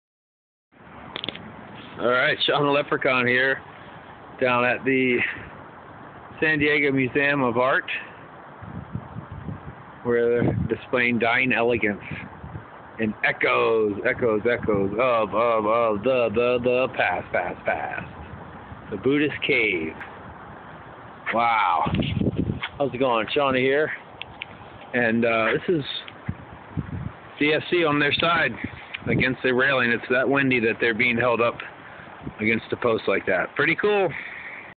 Adopt the DSC - San Diego Museum of Art
Outside of the SD Museum of Art.  It was so windy that my cutout was sticking to the railing.